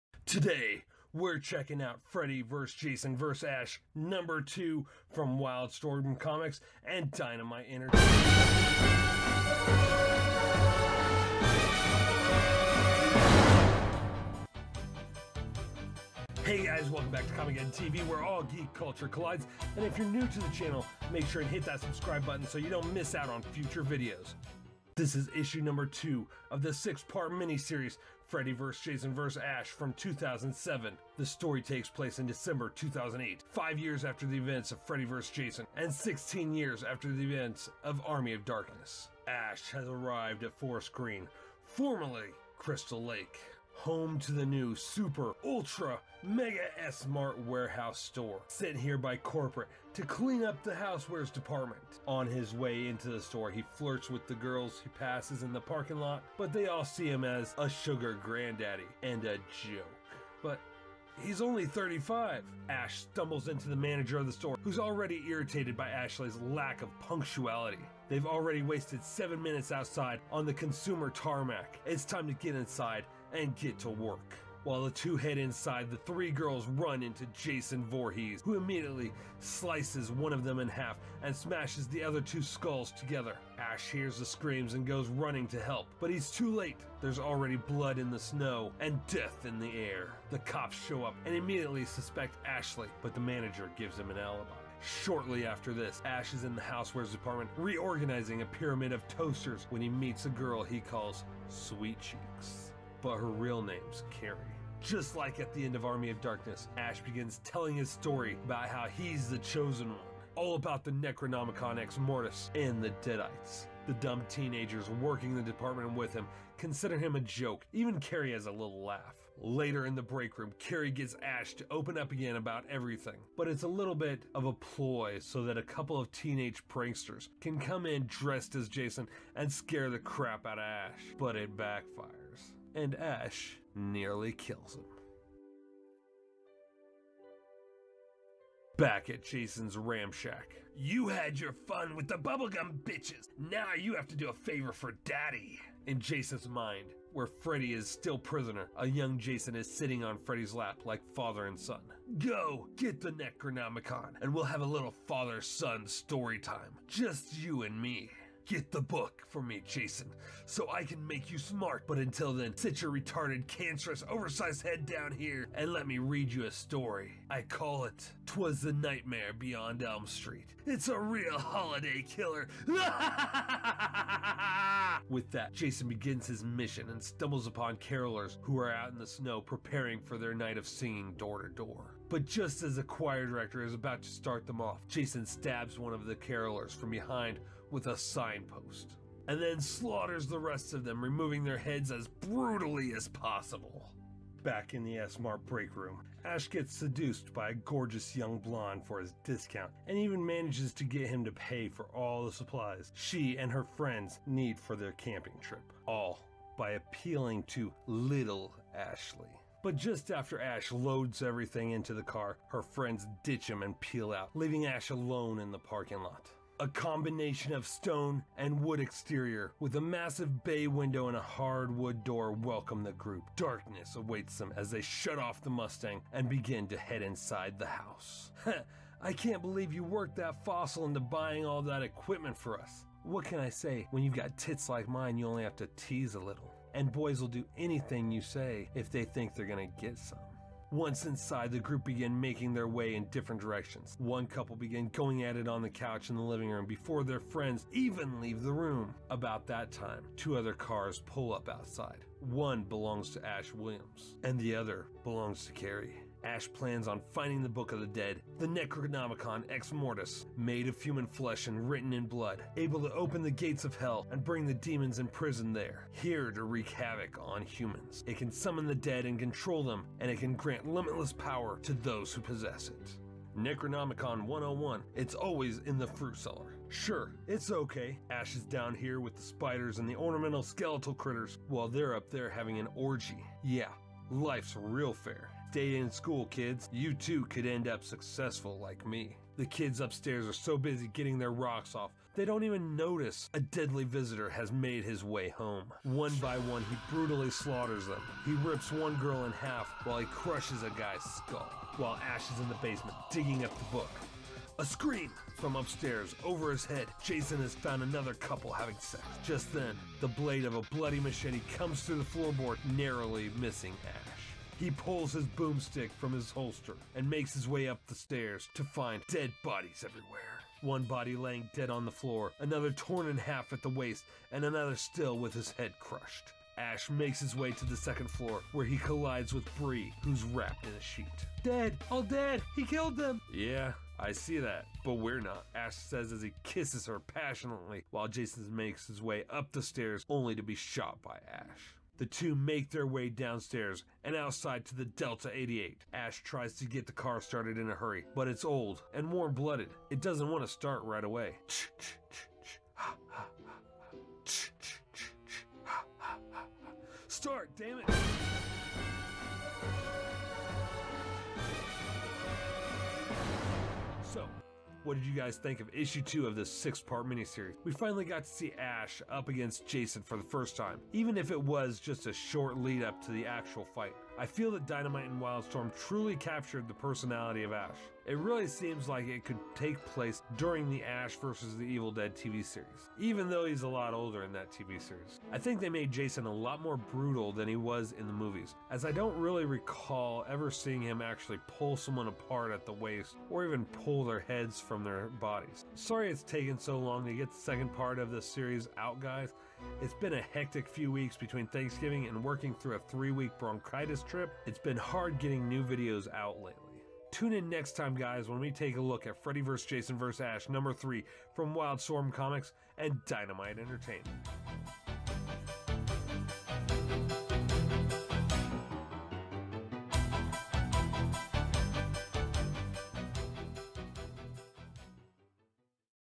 Freddy vs Jason vs Ash Issue #2 Wildstorm/ Dynamite Entertainment 2007 (Ash Williams/Bruce Campbell)
This Video is NOT made for kids, This video is for adult collectors Freddy vs Jason vs Ash Issue #2 Wildstorm/ Dynamite Entertainment 2007 Ash Williams Freddy vs Jason vs Ash Issue #2  Comicgeddon TV Hosted & Created by Shannon Cornthwaite
Freddy vs. Jason vs. Ash is a six-issue comic book limited series that was released in November 2007 and ran until March 2008. It was published by Wildstorm, Dynamite Entertainment, and DC Comics. Based on the original Freddy vs. Jason vs. Ash film treatment by Jeff Katz, the story serves as a sequel to Freddy vs. Jason and The Evil Dead trilogy. The comic book series was written by James Kuhoric and illustrated by Jason Craig.

Comicgeddon TV
Mission Statement: